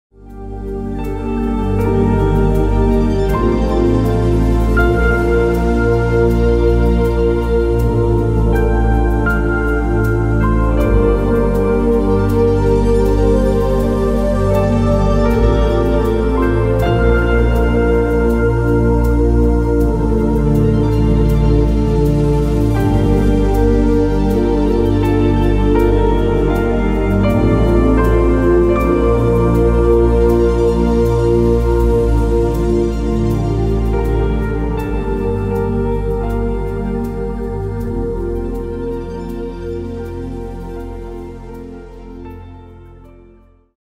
Döttling Gyrowinder
This video gives you a good idea of how the highly exclusive Gyrowinder made by Döttling does look like.